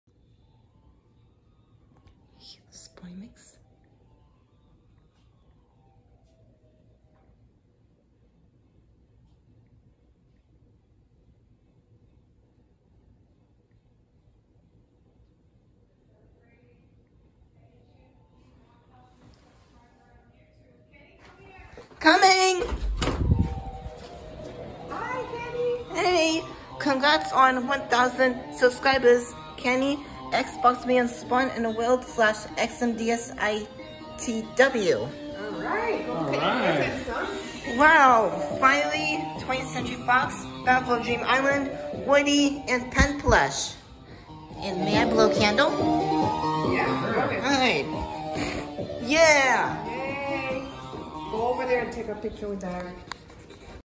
Oct 2, 2021
We Celebrated 1,000 Subs on my channel last night and I got Firey Plush, Pen Plush, Woody Plush, BFDI Logo, and 20th Century Fox